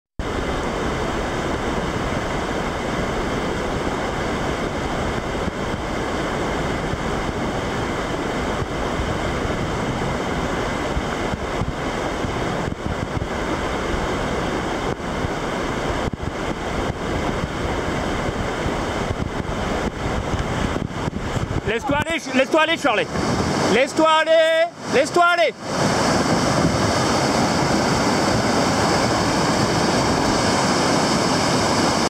Ganière en kayak - Rapide du Martinet - Shirley
Descente de la Ganière (Ardèche) en kayak - plus d'infos - 27 avril 2019